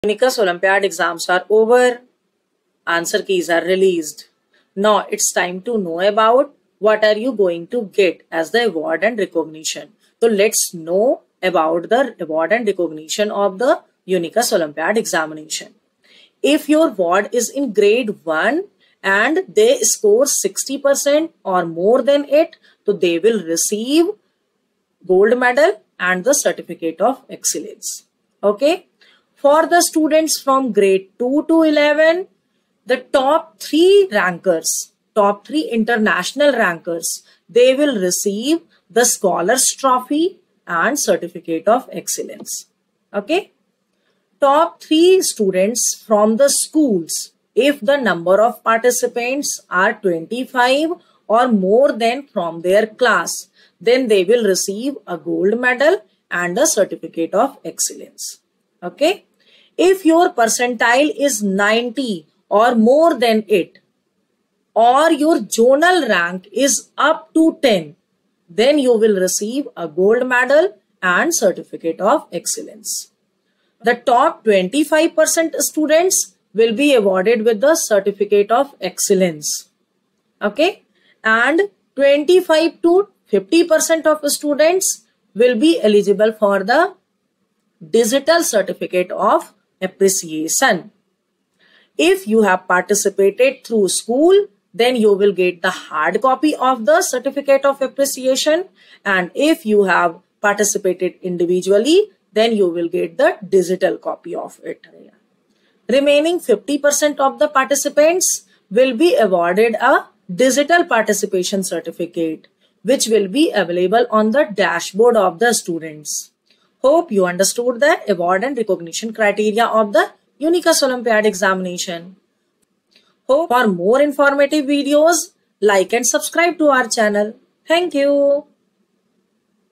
Unicus Olympiad Awards:Criteria and Recognition Explained!
Curious about the award and recognition criteria for Unicus Olympiad exams? Watch this video to learn how students are evaluated and recognized for their achievements. Understand the criteria and inspire your knowledge to strive for excellence!